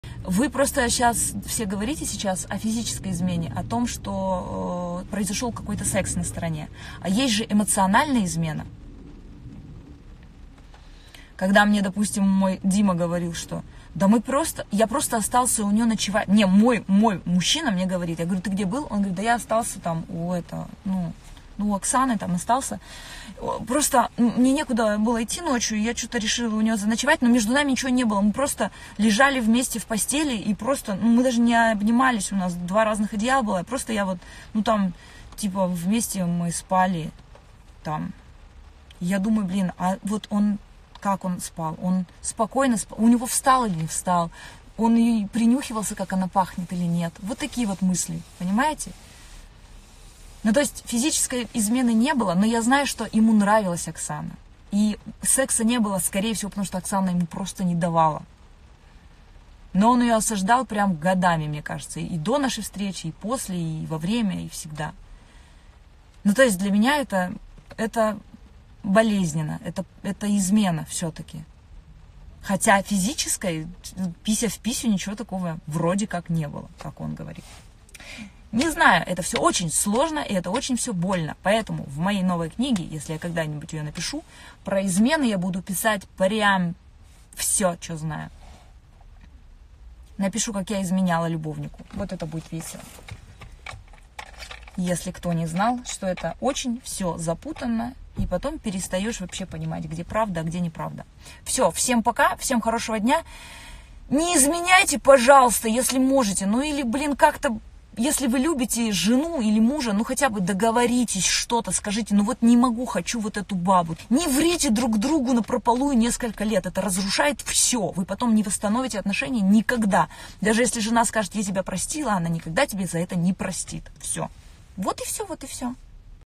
ЭМОЦИОНАЛЬНАЯ ИЗМЕНА
Эмоциональная измена — это когда человек состоит в браке или серьёзных отношениях, но отдаёт кому‑то другому больше времени, энергии и чувств, чем своему партнёру. С этим «другом» он делится сокровенными мыслями, к нему предпочитает обращаться за поддержкой, теплом и эмоциональными поглаживаниями. Иными словами, ищет эмоций на стороне.